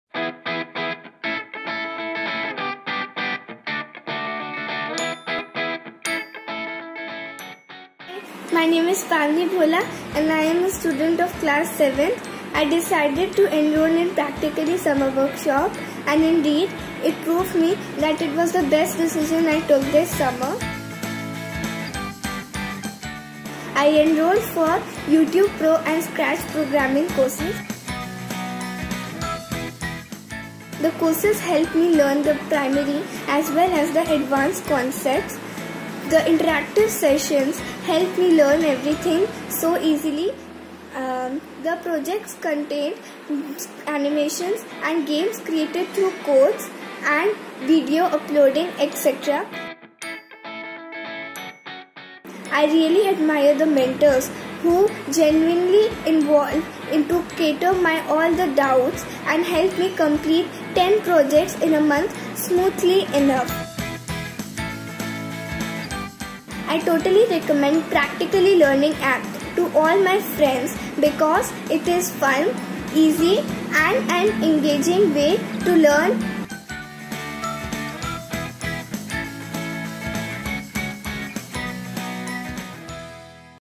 StudentsSpeak ft. Pavani | Testimonial
Let's hear it from our Summer Workshop Champ, Pavani Bhola, as she shares her experience learning YouTube Pro and Scratch Programming with Practically!

Don't just learn, learn Practically!